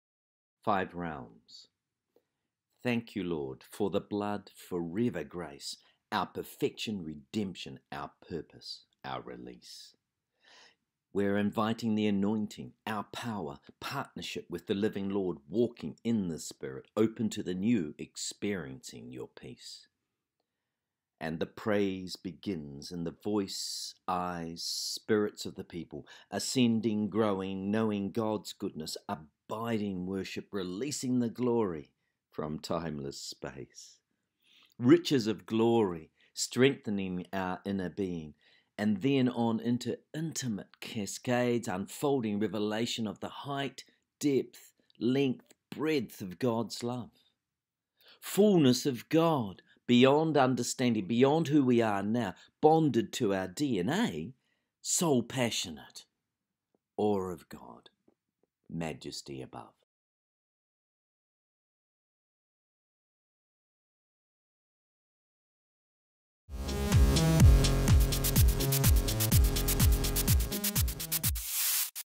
Five Realms
Five Realms of unfolding awareness of God towards true, deep revival. Christian Poetry from matrixmoments.com. Questions, thoughts and moments in spoken word.